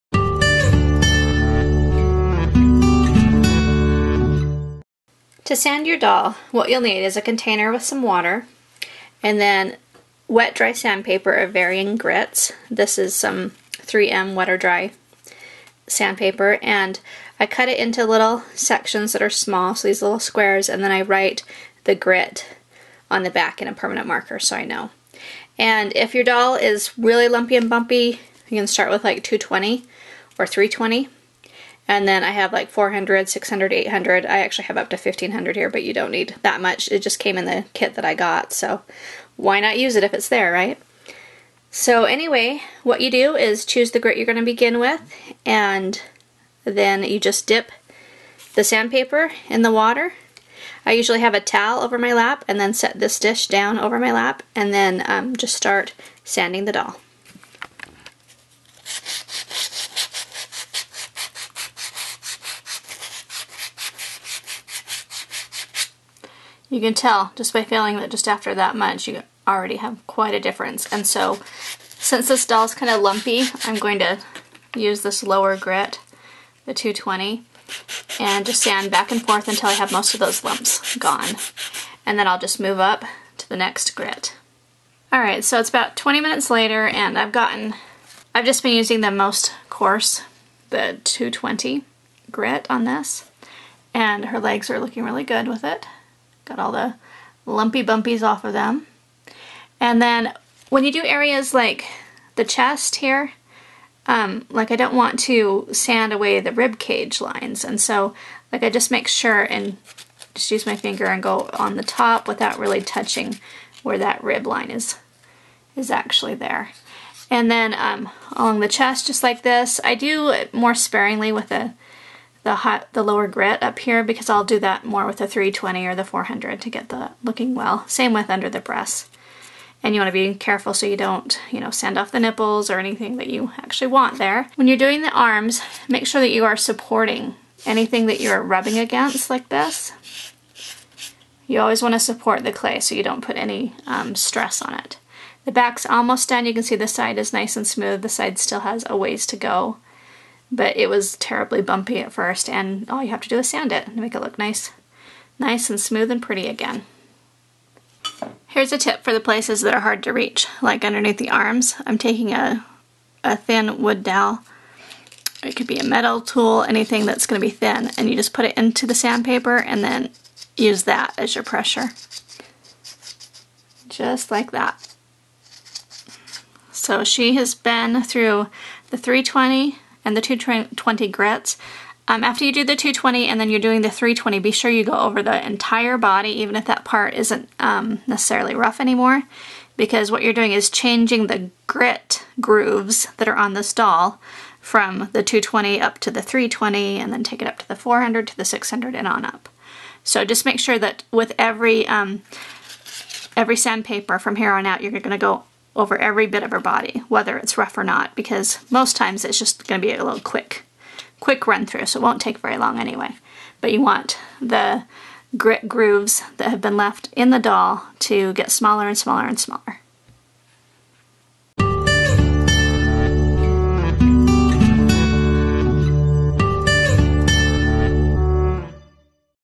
Tips on Sanding your One of a Kind Art Doll by Aprylian - SculptUniversity.com
to sign up for my free Tips & Tricks Newsletter and for DVDs.
This doll is from the new sculpting video set Sculpting a Female Fairy, available in Feb 2014.  The sandpaper is 3M Wet/Dry